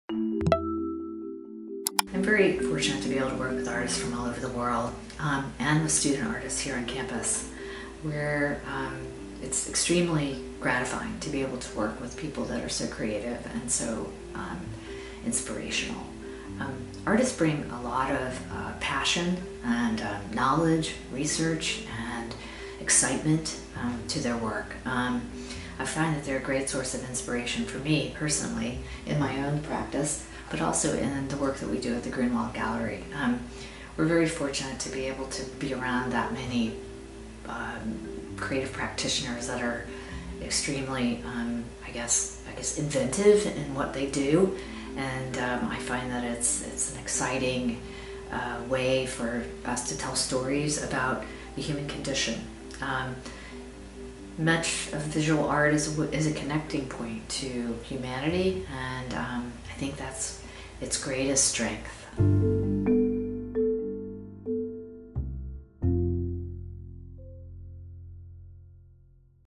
Betsy Stirratt | Grunwald Gallery of Art
Our campus leaders reflect on the importance of the arts and humanities in building, sustaining, and strengthening community, especially in times of great challenge and uncertainty.